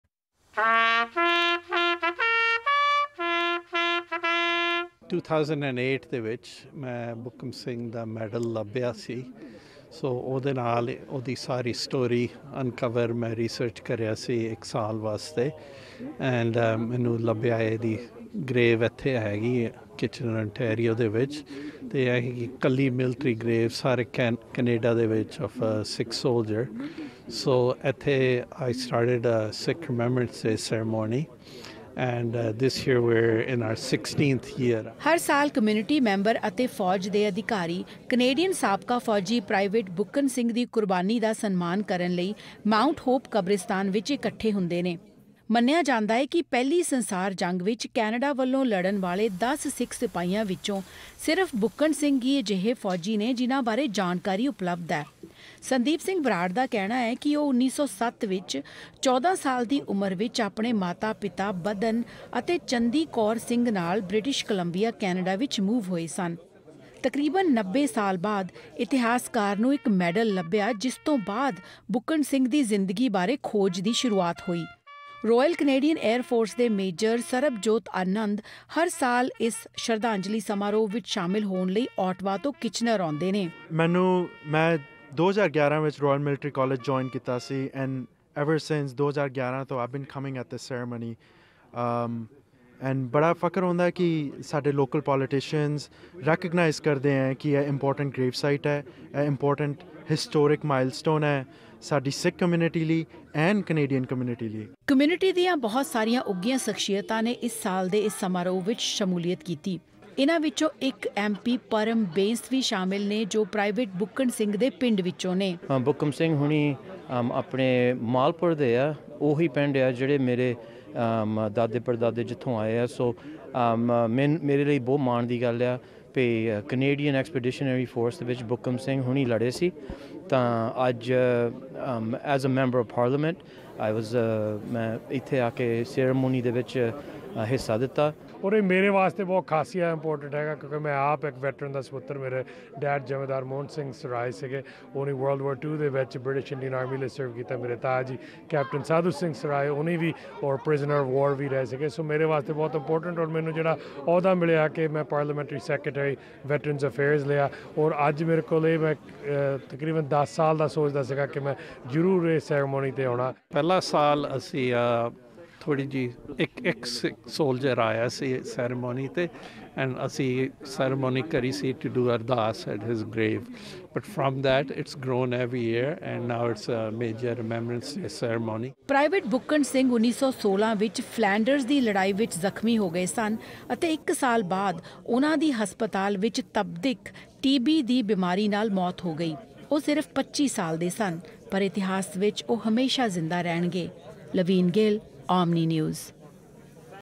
16th annual Sikh Remembrance Day Ceremony in Kitchener | OMNI News Punjabi
ਮੰਨਿਆ ਜਾਂਦਾ ਹੈ ਕਿ ਪ੍ਰਾਈਵੇਟ ਬੁੱਕਣ ਸਿੰਘ ਕੈਨੇਡਾ ਦੀ ਫੌਜ ਵਿੱਚ ਪਹਿਲੇ ਅਜਿਹੇ ਫੌਜੀ ਰਹੇ ਸਨ ਜਿਨਾਂ ਬਾਰੇ ਕੌਮੂਨਿਟੀ ਜਾਣੂ ਹੋਈ। ਪ੍ਰਾਈਵੇਟ ਬੁੱਕਣ ਸਿੰਘ ਨੂੰ ਸ਼ਰਧਾਂਜਲੀ ਦੇਣ ਲਈ ਕੱਲ ਦੇਸ਼ ਭਰ ਤੋਂ ਆਏ ਹੋਏ ਅਧਿਕਾਰੀ ਅਤੇ ਕੌਮੂਨਿਟੀ ਮੈਂਬਰਜ਼ ਕੱਲ ਕਿਚਨਰ ਵਿੱਚ ਇਕੱਠੇ ਹੋਏ |